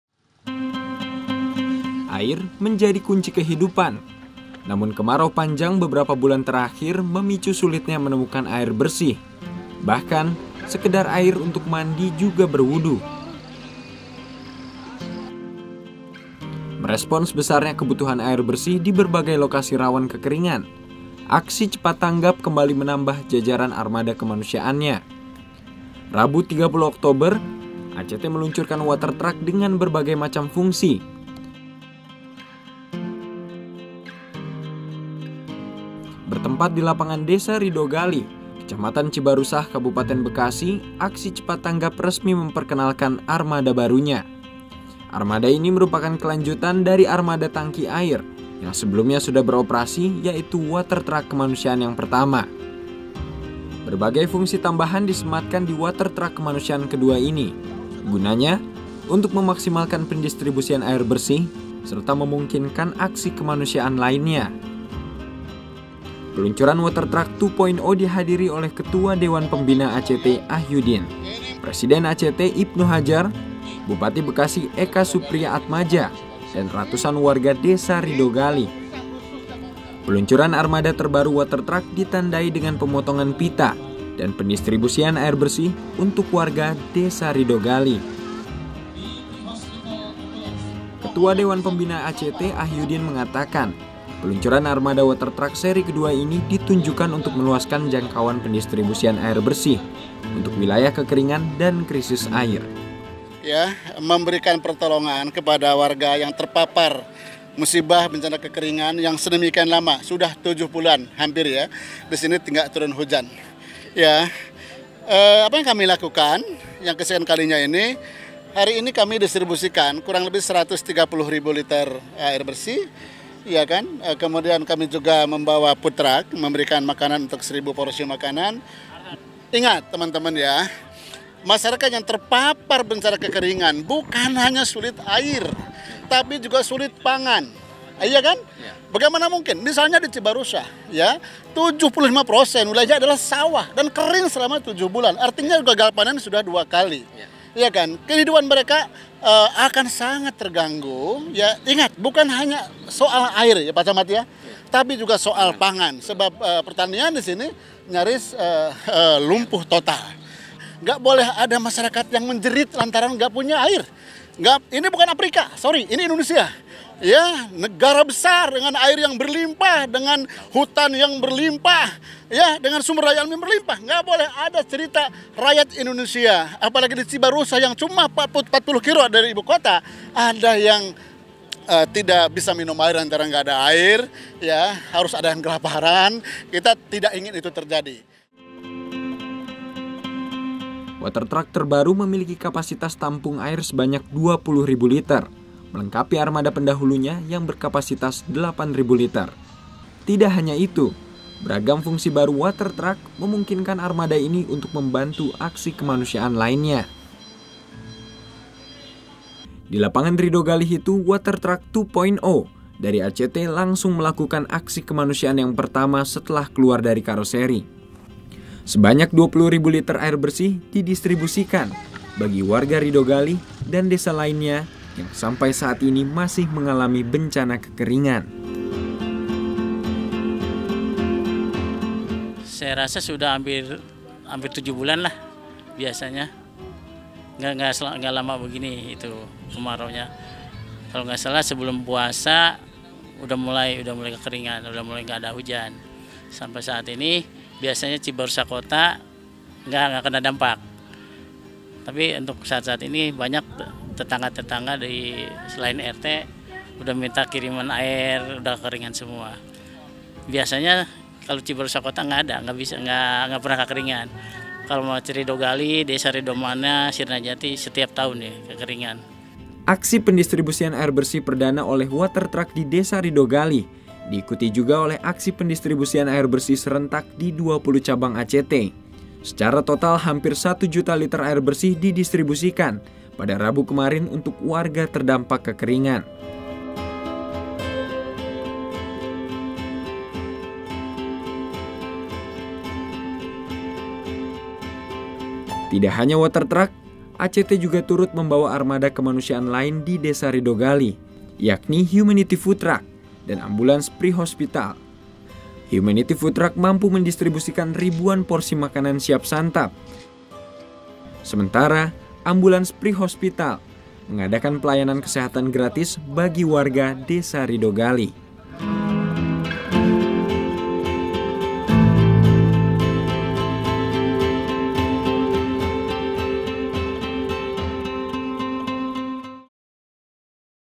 WATER TRUCK 2.0 Siap Bergerak Mendistribusikan AIR BERSIH GRATIS
Bertempat lapangan Desa Ridogalih, Kecamatan Cibarusah, Kabupaten Bekasi, Aksi Cepat Tanggap (ACT) resmi memperkenalkan armada barunya, Rabu (30/10). Armada ini merupakan kelanjutan dari armada tangki air yang sebelumnya sudah beroperasi, yaitu Water Truck. Berbagai fungsi tambahan diberikan, gunanya untuk memaksimalkan pendistribusian air bersih serta memungkinkan aksi lain.

Ketua Dewan Pembina ACT Ahyudin mengatakan, peluncuran armada Water Truck seri kedua ini ditujukan untuk meluaskan jangkauan pendistribusian air bersih untuk wilayah kekeringan dan krisis air. Water Truck menjadi salah satu solusi yang dikeluarkan ACT untuk mengatasi kebutuhan air masyarakat, selain Sumur Wakaf yang telah dibangun di berbagai penjuru negeri.

Water Truck terbaru memiliki kapasitas tampung air sebanyak 20 ribu liter, melengkapi armada pendahulunya yang berkapasitas 8 ribu liter. Tidak hanya itu, beragam fungsi baru Water Truck memungkinkan armada ini untuk membantu aksi kemanusiaan lainnya.

Sutaryo selaku Koordinator Water Truck mengatakan, Water Truck dari ACT ini memiliki tiga keluaran air bertekanan serta dua keluaran air memanfaatkan gravitasi. Mesin keluaran air terletak di bagian paling belakang truk air. Keluaran air bertekanan ini memanfaatkan putaran mesin sehingga memungkinkan untuk penyemprotan air dalam tekanan tinggi, yakni 3 bar hingga 7 bar.

Dengan dilengkapi dengan mesin keluaran air, fungsi Water Truck tak hanya sebagai pendistribusi air bersih saja bagi warga yang membutuhkan. Armada tersebut juga dapat digunakan untuk membantu pemadaman api.